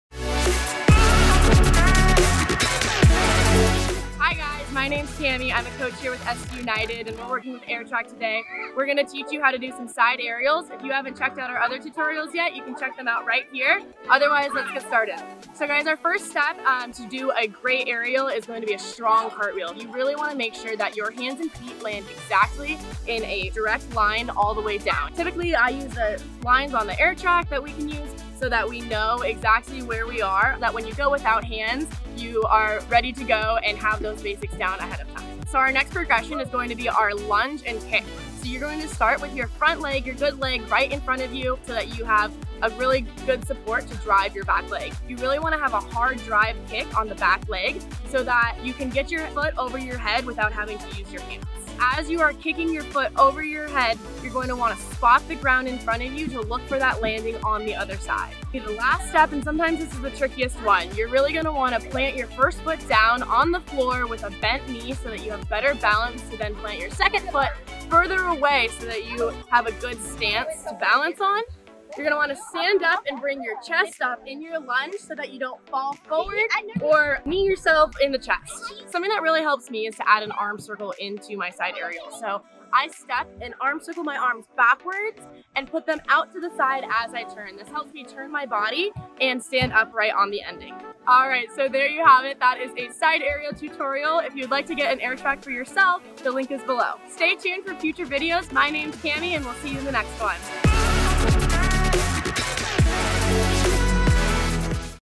How to: SIDE AERIAL Beginner Tutorial | AirTrack™
If you want to upgrade your Cartwheels, try doing one without using your hands! Here's a tutorial that breaks down the steps in order to learn and practice a Side Aerial!

Time Stamps:
0:00 Intro
0:15 Cartwheel
0:35 Lunge and Kick
0:52 Spotting the Ground
0:59 Landing and Sticking
1:18 Helpful Tip
1:31 Outro